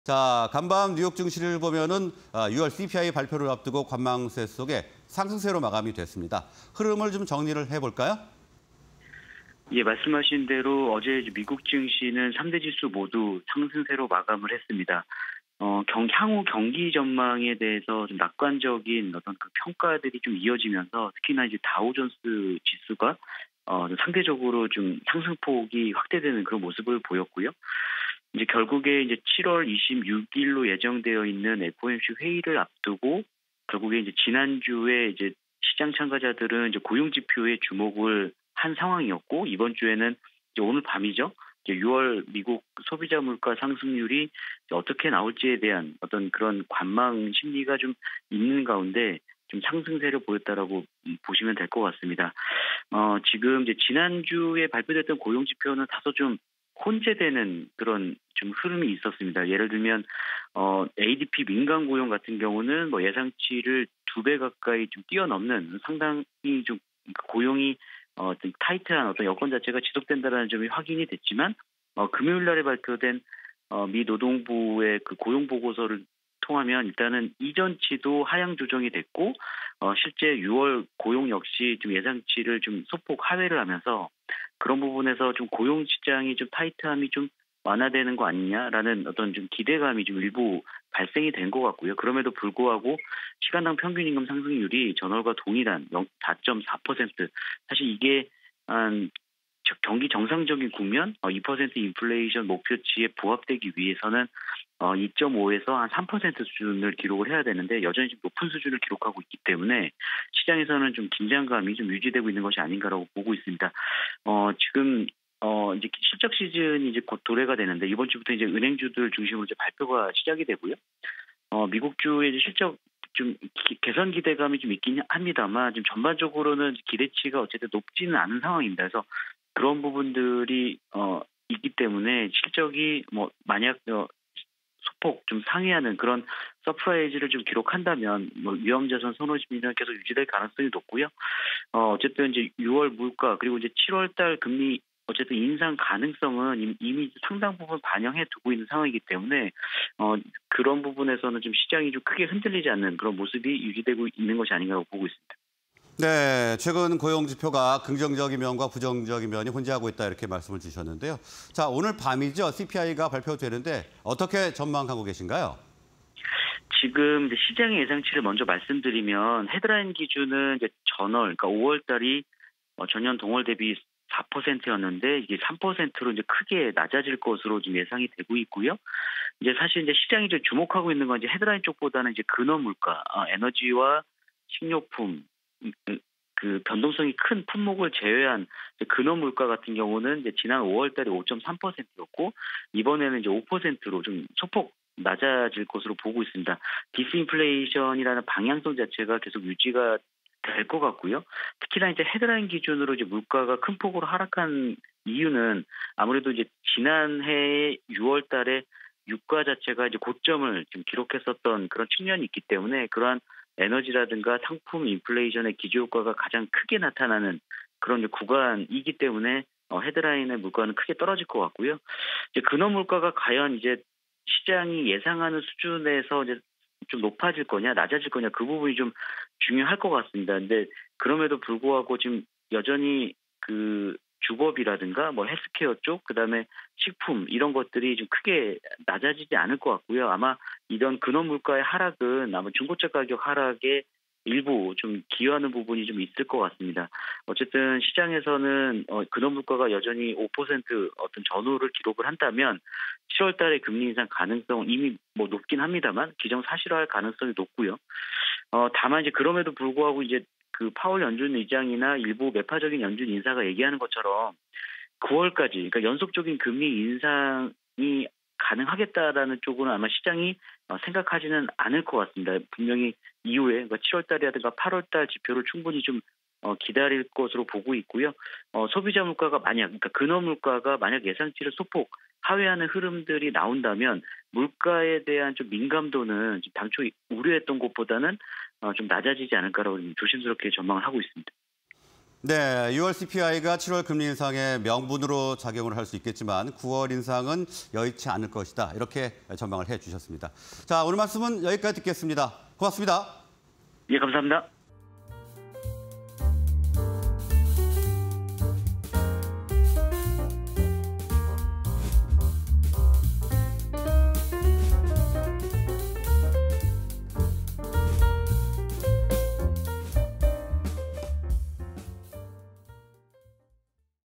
美증시, 조정이냐 랠리냐… 호재와 악재는?_투자 인사이트 (20230712)
Q. 美증시, 조정이냐 랠리냐… 호재와 악재는?
Q. 美 금리 인상 속도 가를 'CPI'… 전망은?